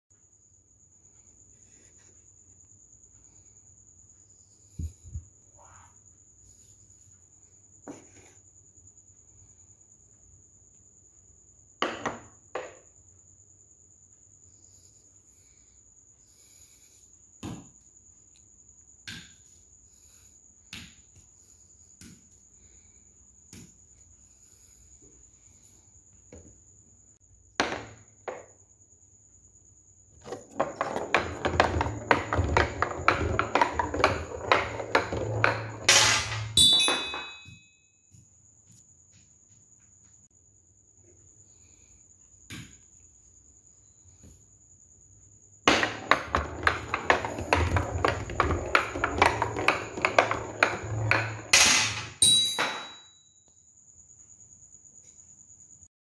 Marble Run Race ASMR, Who Will Win? Any Guesses, Lighting VS Blue Ball
Today i made a video of Balls Race, Lighting VS Blue Marble